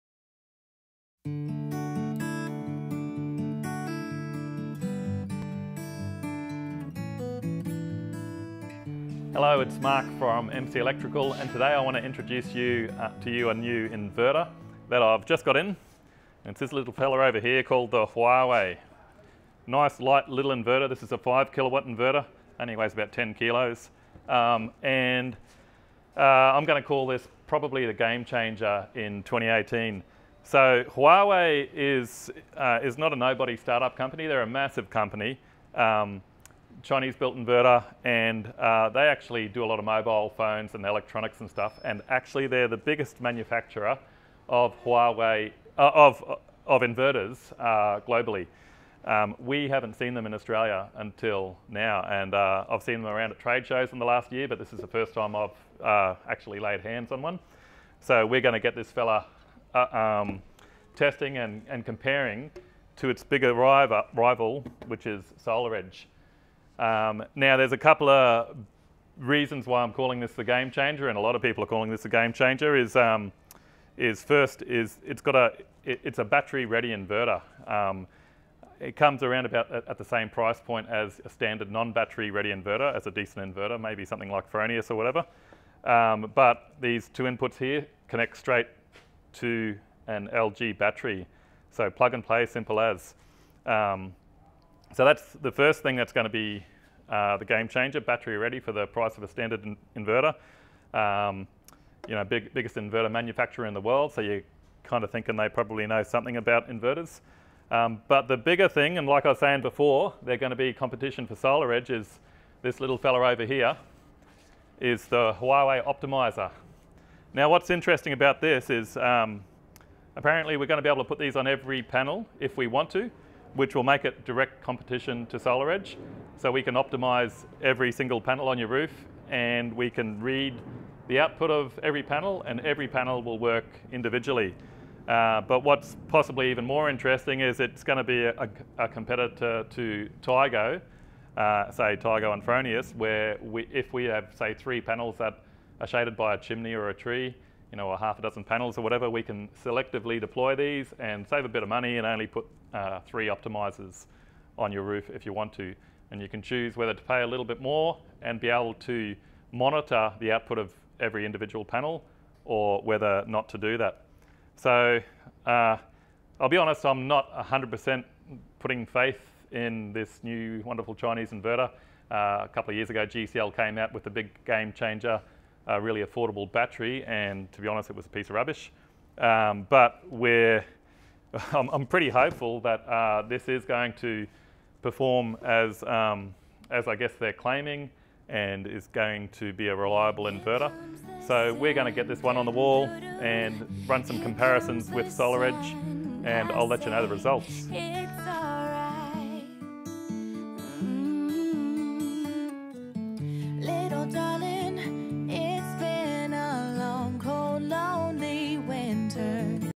Huawei Intro V4 sml
Mark introduces the battery ready Huawei inverter.

☀️ Looking to go solar in South-East Queensland? We're happy to help!
🌏 It's great for the planet, and even better for your wallet.

🇦🇺 Brisbane, Australia